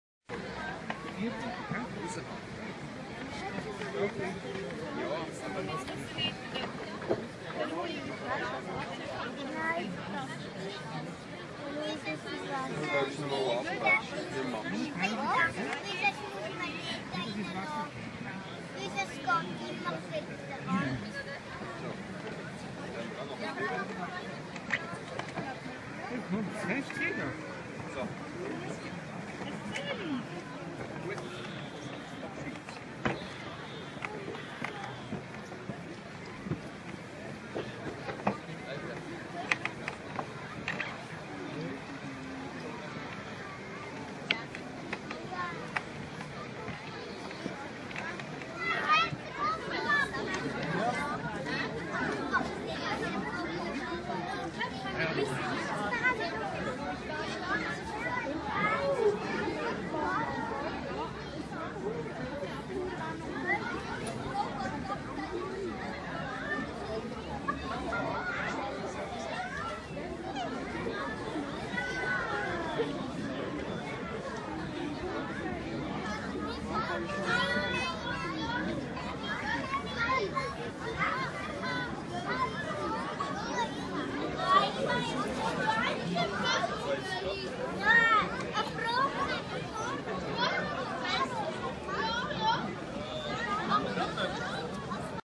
Lichterschwemmen
Lichterschwemmen in Ermensee am Donnerstag, 6. März 2014.